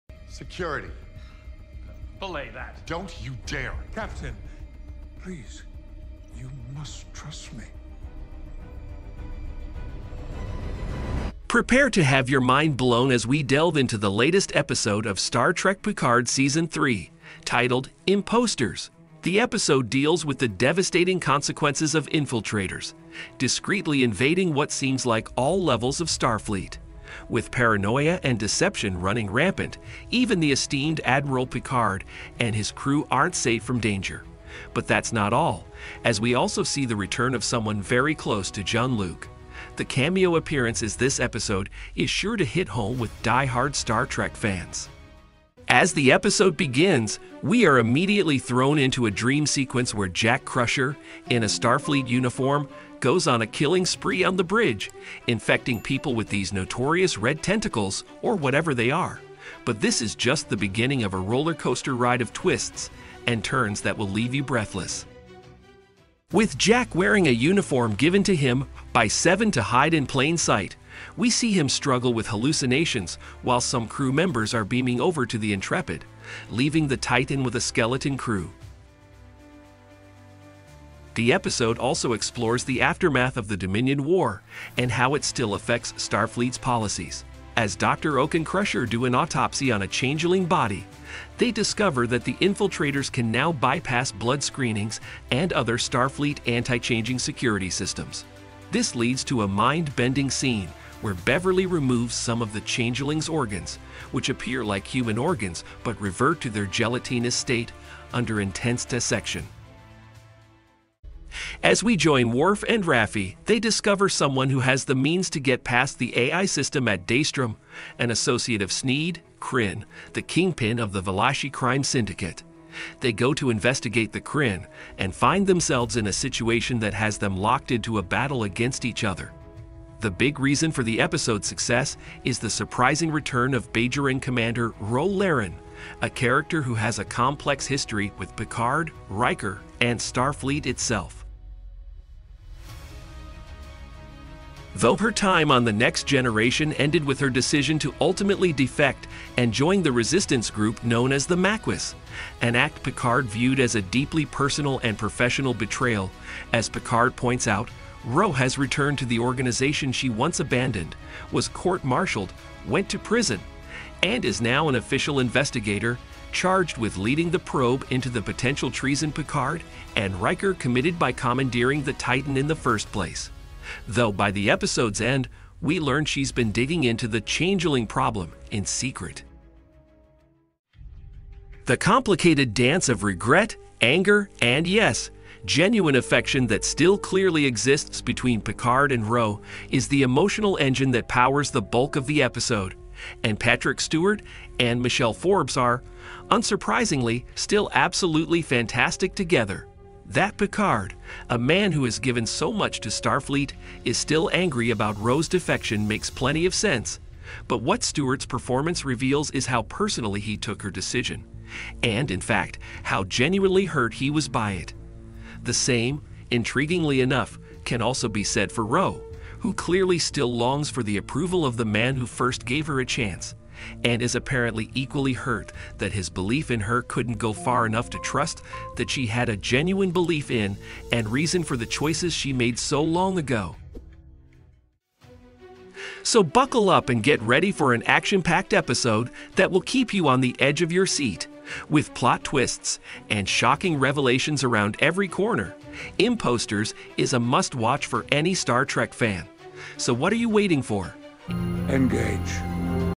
Star Trek Picard S3 Ep5 "Impostors" QUICK overview
A quick rundown of the last episode of ST: Picard "Impostors"